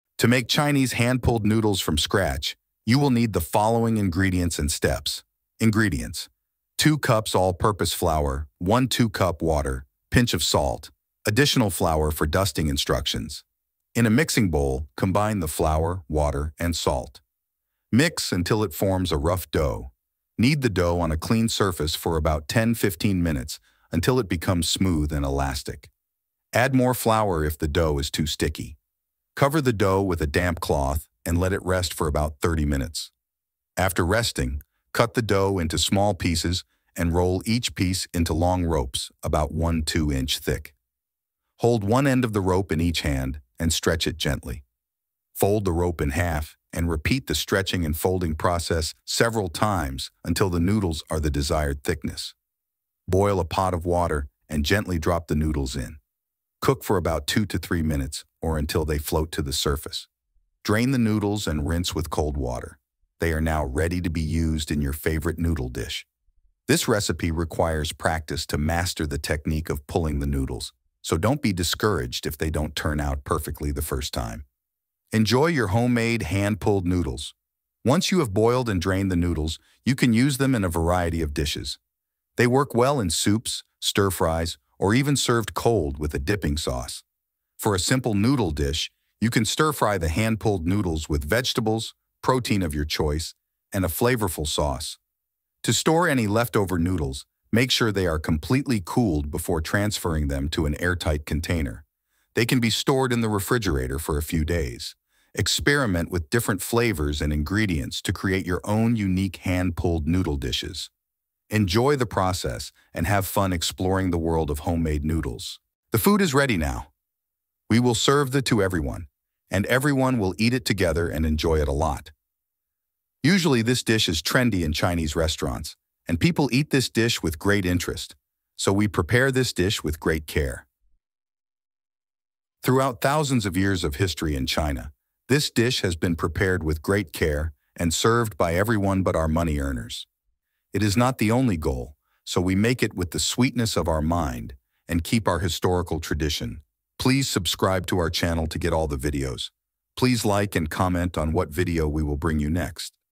Chines hand MADE noodles recipe | 100 Years History
making hand-pulled noodles, you can use the following template:

Title: "Homemade Hand-Pulled Noodles Recipe | Mastering the Art of Noodle Making"

Description:
In this video, we'll guide you through the mesmerizing process of creating authentic hand-pulled noodles from scratch. Watch as we knead, stretch, and pull the dough to craft these delicious noodles that are a staple in Chinese cuisine.

Follow along as we share our step-by-step instructions, tips, and tricks to help you perfect the art of noodle pulling. Whether you're a seasoned chef or a novice in the kitchen, this recipe is sure to impress your taste buds and elevate your cooking skills.

Chinese hand MADE noodles recipe | 100 Years History

Join us on this flavorful journey as we dive into the world of handmade noodles. Let's get cooking!